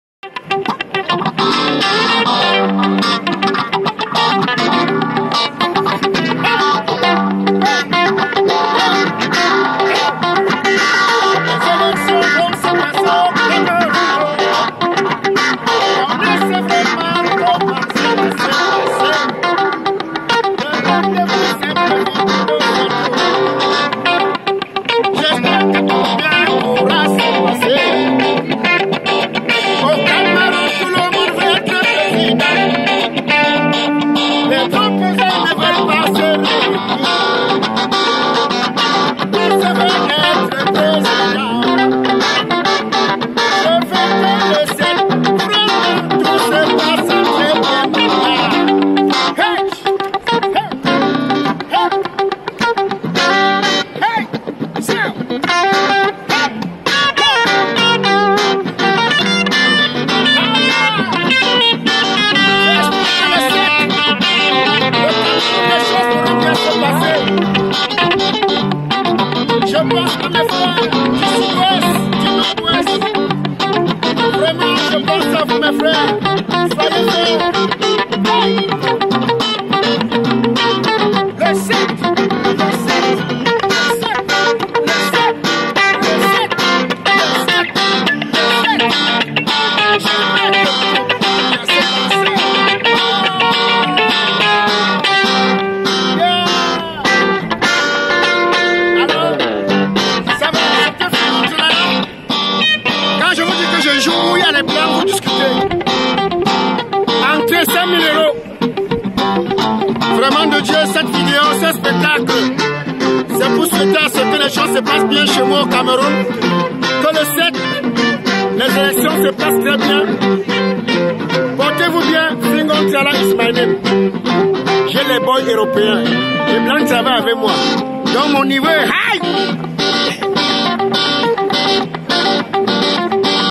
FINGON TRALALA "Motion de soutient pour les elections"
FINGON TRALALA soutient les elections présidentielles à sa façon